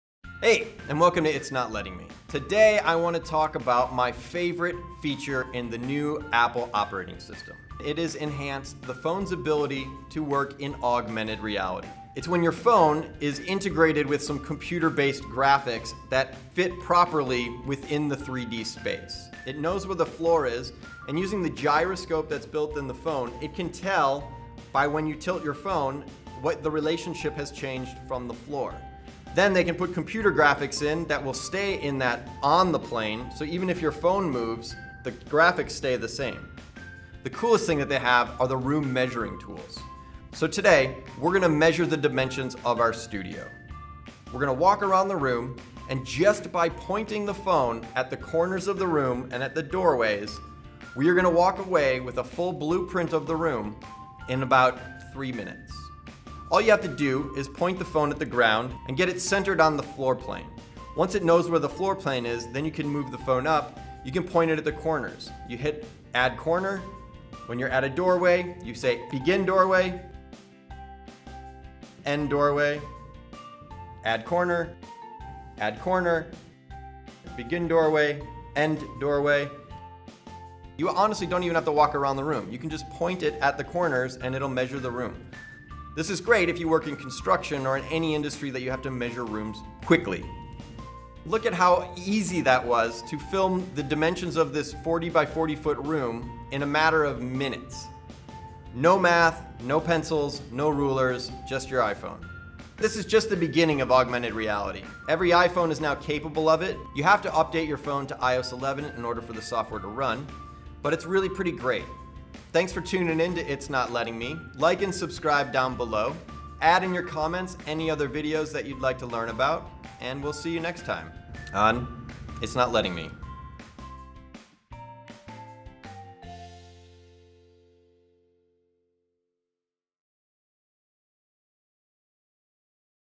How to measure a room with an iphone
🧠 Welcome to It’s Not Letting Me – your guide to mastering tech one tap at a time!

In this episode, Matthew explores the mind-blowing Augmented Reality (AR) tools introduced in iOS 11 and how they’re quietly changing the way we interact with our devices—and the world around us. Whether you're a teacher, creator, or just AR-curious, this video breaks down the most practical and powerful AR apps now available on iPhones and iPads.

🔍 Featured Apps & Tools:

📐 Magicplan – Turn any room into a floor plan just by scanning with your phone. Great for interior design, real estate, and home renovation.

📏 PLNAR – Capture room dimensions and layouts with AR. Export professional CAD-ready files instantly.

👻 Snapchat – Dive into playful AR filters and lenses that bring faces and environments to life. It's more than selfies—it's storytelling in augmented space.

🛋 IKEA PLACE – Virtually place true-to-scale furniture in your home before you buy it. See how that couch really fits before you break your back carrying it upstairs.

Matthew shows you exactly how each of these works and offers insight into the real future of AR—beyond games and gimmicks.

🎯 Whether you're a tech newbie or a seasoned creative, these tools can help you:

Save time on design and layout

Preview purchases

Enhance storytelling and classroom engagement

Better understand the potential of AR as it grows